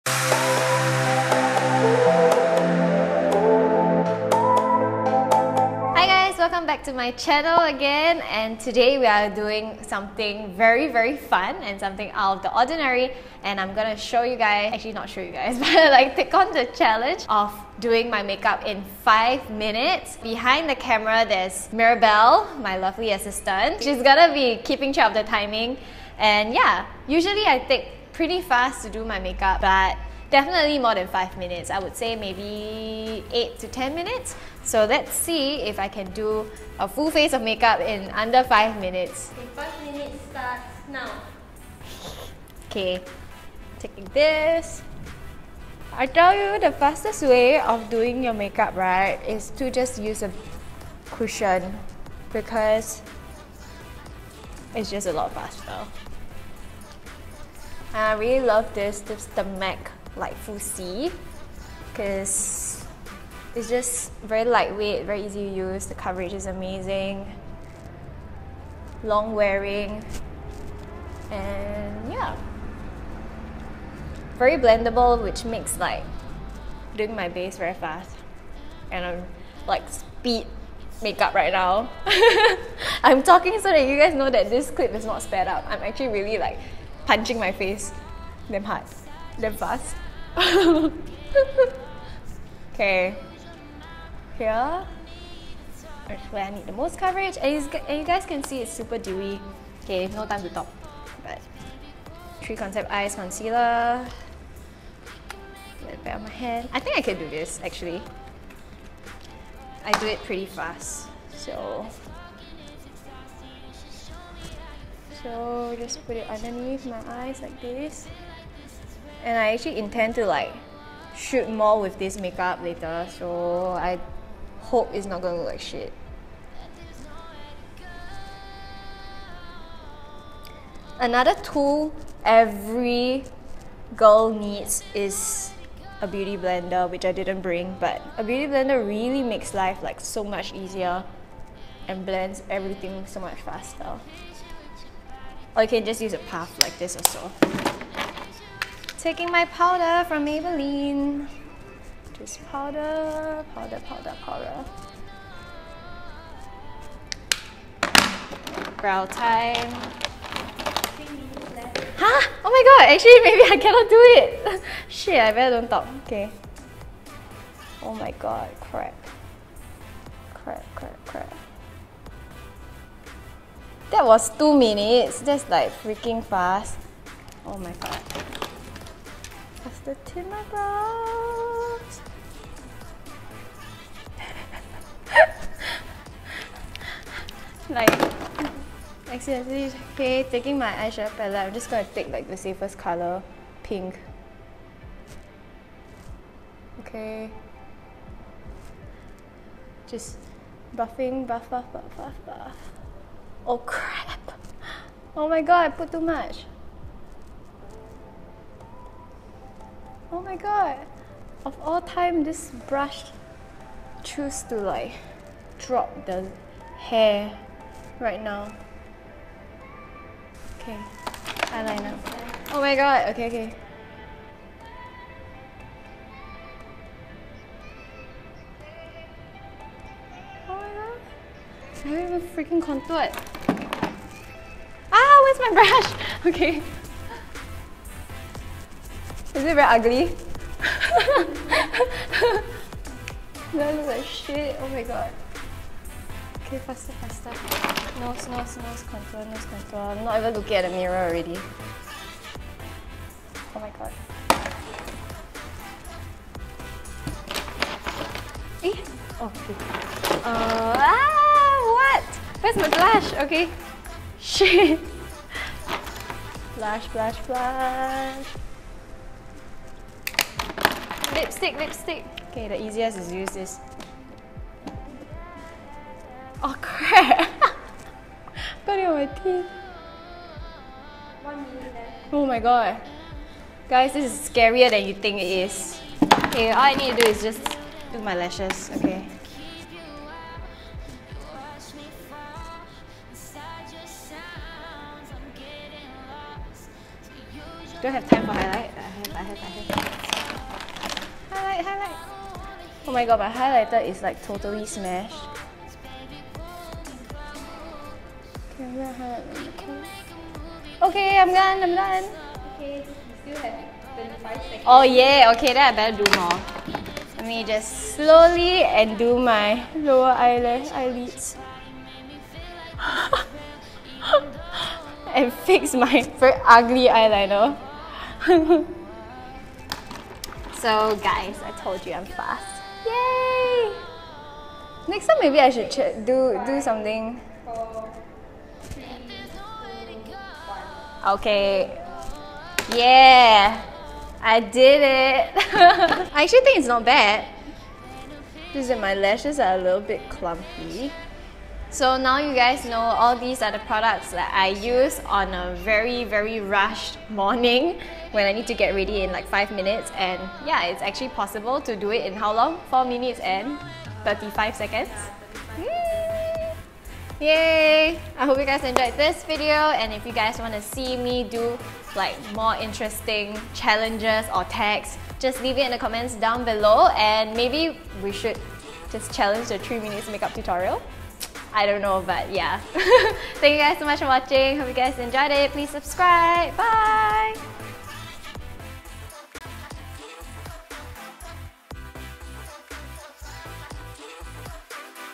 5 MINS MAKE UP CHALLENGE | MONGABONG
Let me know if you think I should challenge 3 mins next :O

PRODUCTS USED:
- MAC Lightful C Cushion - Light Plus
- 3CE Waterful Concealer - 01
- Maybelline Dream Satin 2 way Foundation Cake
- IT Cosmetics Brow Power
- Etude House Color My Brow Brow Mascara - 05
- Anastasia Beverly Hills Modern Renaissance Eyeshadow Palette
- Clio Professional Gelspresso Waterproof Gel Liner 04
- Tarte Cosmetics 24H Amazionian Clay Blush - Exposed
- Benefit Cosmetics Hoola Bronzer
- Etude House Color In Juicy Lip Tint OR212